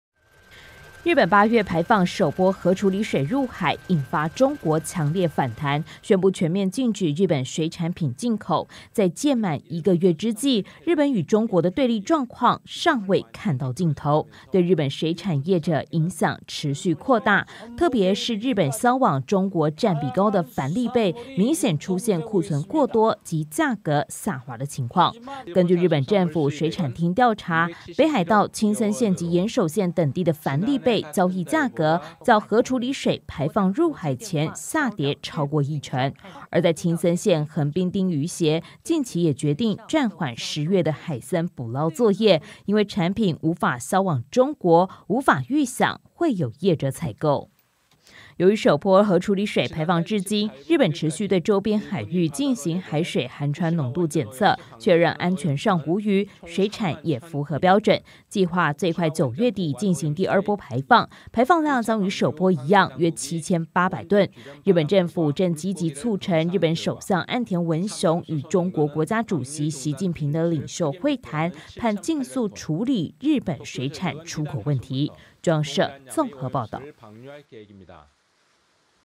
日本核處理水排海 衝擊水產出口量｜中央社影音新聞
日本8月將首波「核處理水」排放入海，引發中國強烈反彈，如今屆滿一個月，中國仍持續抵制日本水產。由於北海道在2022年出口的水產，有超過6成銷往中國，在禁令之下已出現庫存過多及價格下滑的情況。